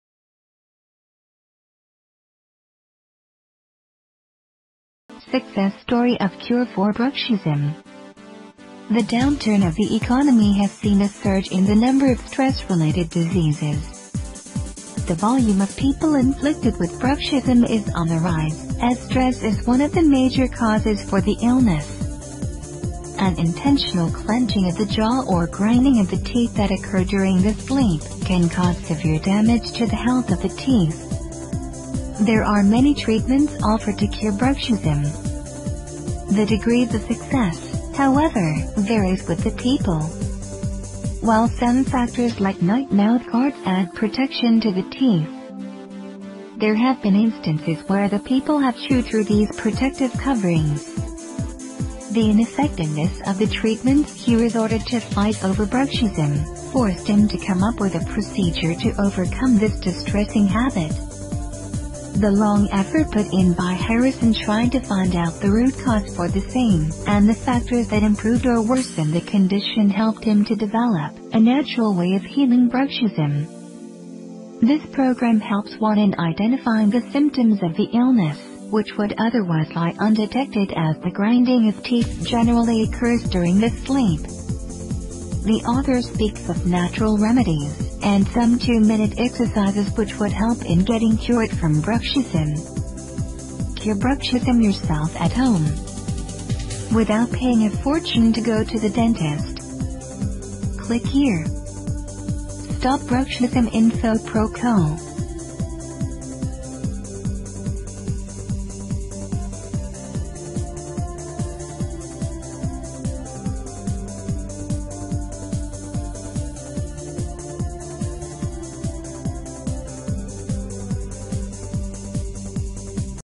Bruxism Symptoms, How To Stop Teeth Grinding, Grinding Teeth Guard, Symptoms Of Teeth Grinding
Fighting bruxism
Statistics reveal that millions are suffering from bruxism which is the involuntary clenching or grinding of teeth that generally occur during the sleep.
Clenching the teeth with immense force or grinding them on a continuous basis can cause permanent damage to the gums and teeth
as it would result in the crushing through the protective shield created by the enamel, thus damaging the roots.

Bruxism is a disease which is not recognized by many until irreversible damage has been made to the teeth which might result in a huge dental expenditure if not detected on time.

Identifying the signs of bruxism is the first step involved. Some of the common symptoms are cited below:

Morning migraines

Painful jaws

Insomnia

Teeth oversensitive to cold, hot and citrus

Regular headaches

Restless sleeps during the night

Chipping of teeth

truth about bruxism stop grinding your teeth tonight. click here.